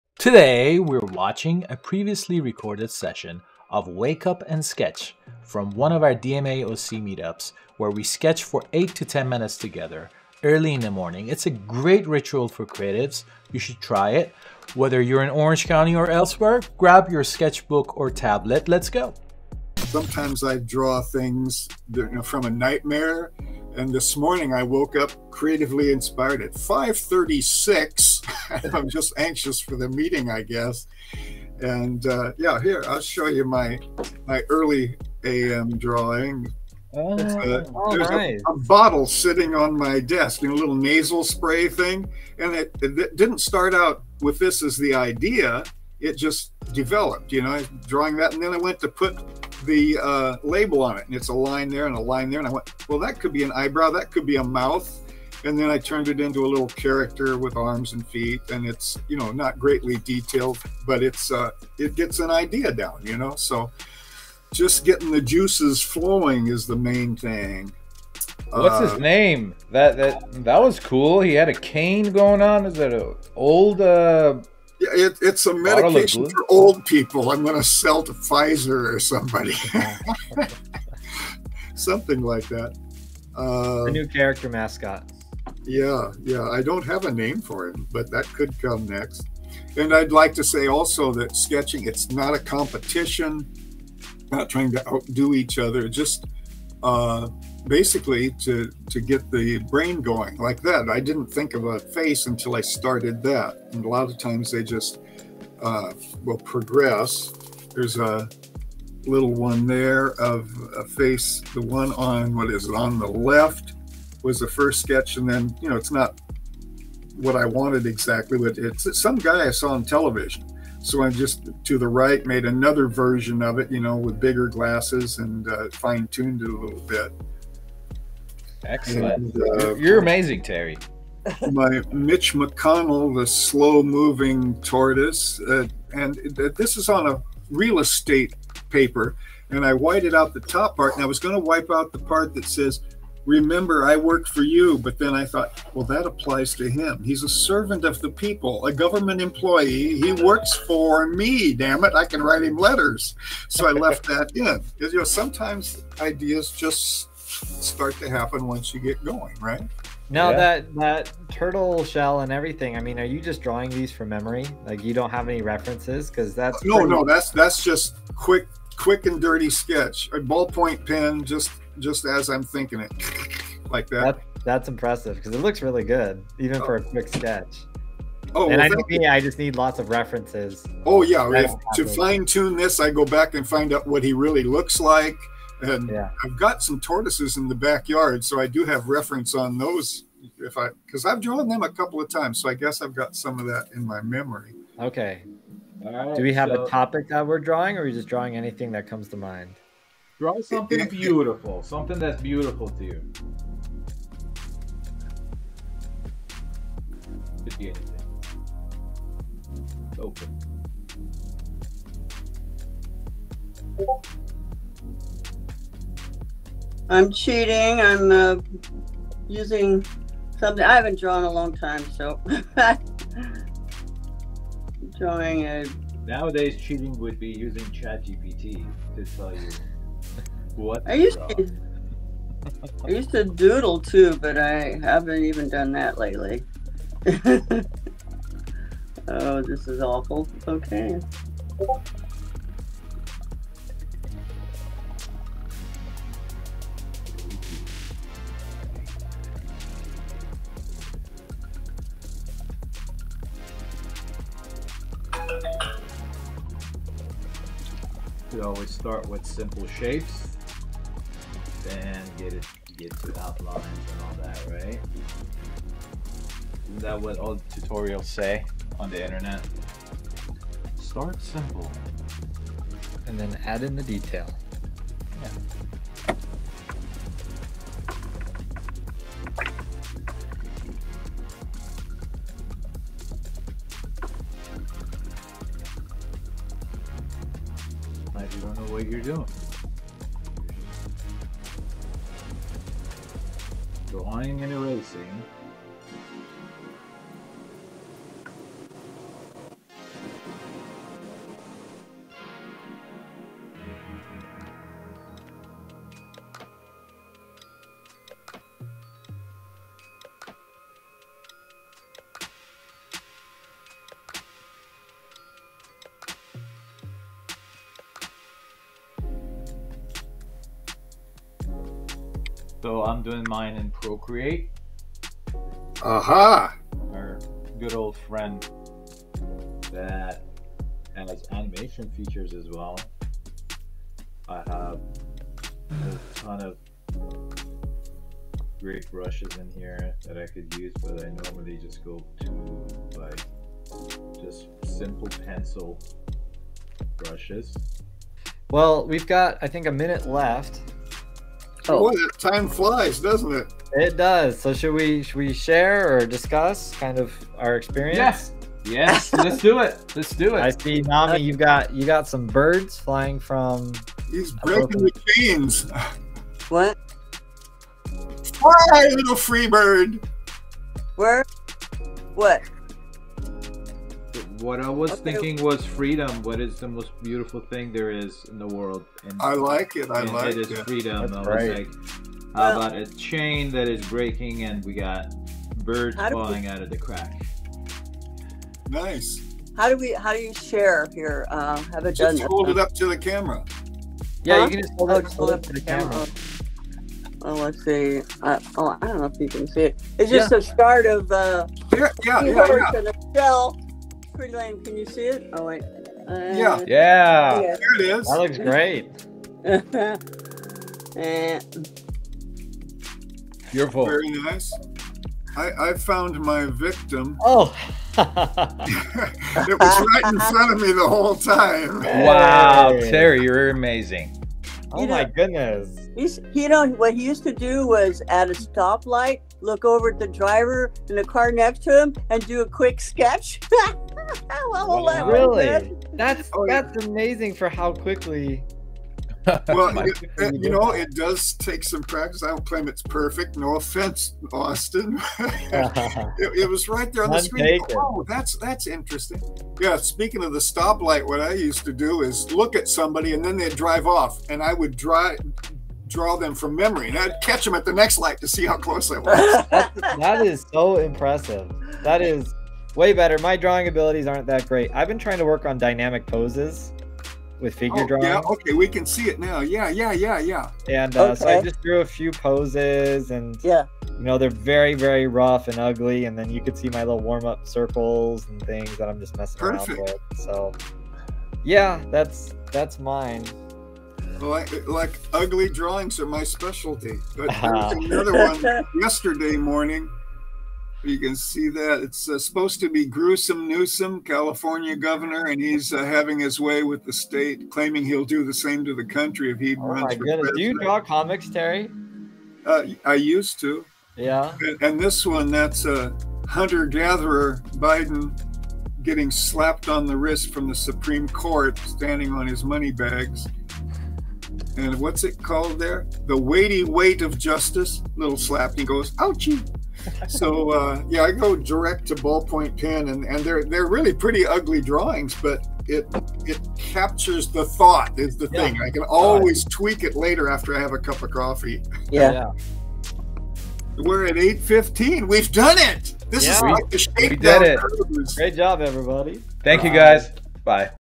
Wake Up & Sketch: 15-Minute Morning Boost for Artists
Start your day with creativity! Join us for a 15-minute 'Wake Up & Sketch' session to get your artistic juices flowing. Perfect for artists of all levels, this quick meetup will help you kickstart your creative day.